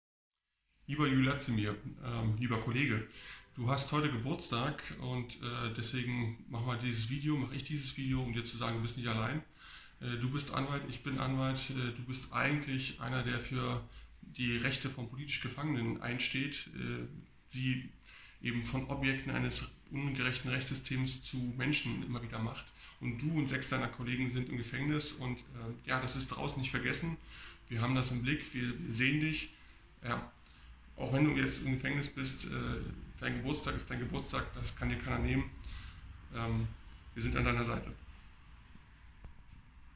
[BY&EN SUB] Штэфан Гельбхаар Уладзіміру Лабковічу: "Мы разам з вамі!"
Дэпутат нямецкага Бундэстага Штэфан Гельбхаар, які ў межах кампаніі WeStandByYou з'яўляецца сімвалічным "хросным" праваабаронцы Уладзіміра Лабковіча, павіншаваў "вясноўца" з Днём народзінаў.

English

Stefan Gelbhaar, a member of the German Bundestag and the godparent of human rights defender Uladzimir Labkovich as part of Libereco's WeStandByYou campaign, congratulated the imprisoned Viasna member on his birthday.